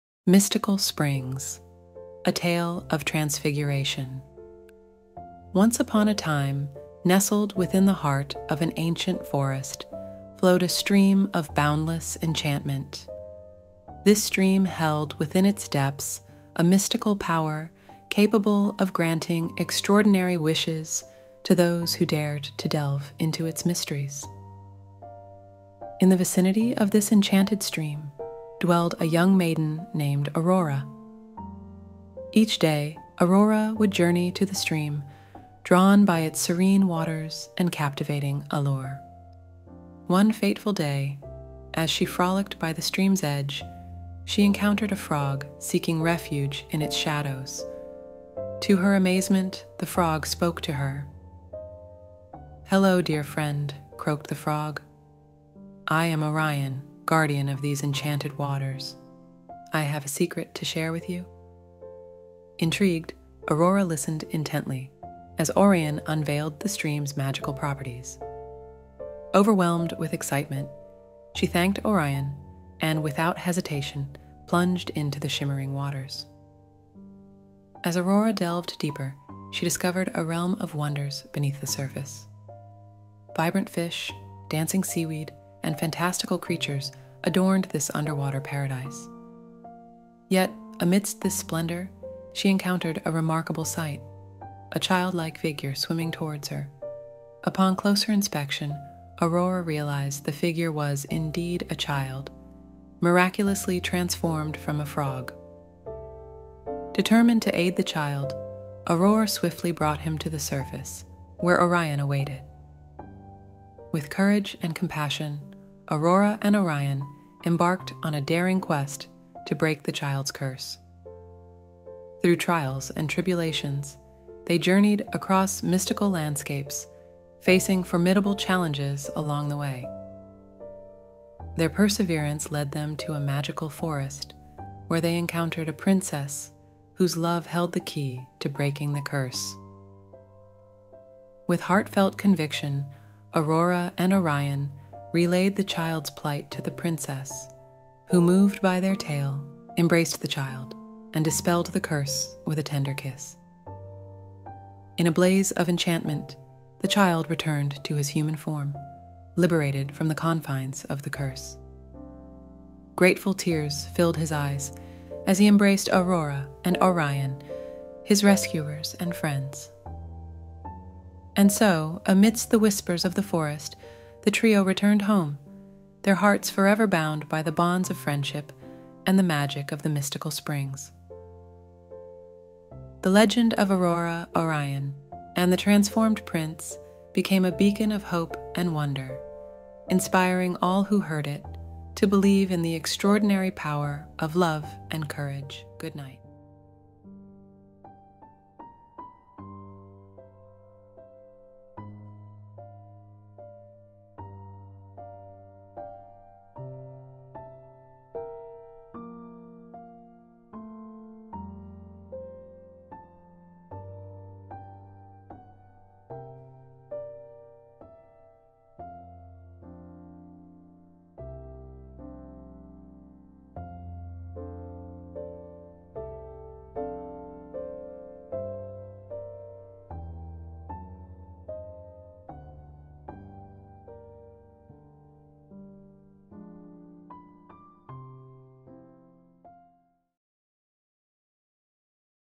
Bedtime Story for Kids! ~~ Mystical Springs:A Tale of Transfiguration 🐸 🫅 🐠
In the heart of an ancient forest lies a stream of boundless enchantment, guarded by Orion, its mystical guardian. Aurora, a young maiden, discovers the stream's magical properties and encounters a child transformed from a frog. With courage and compassion, Aurora and Orion embark on a daring quest to break the child's curse. Their journey leads them to a princess whose love holds the key to dispelling the curse, ultimately uniting them in a tale of friendship, courage, and the extraordinary power of love.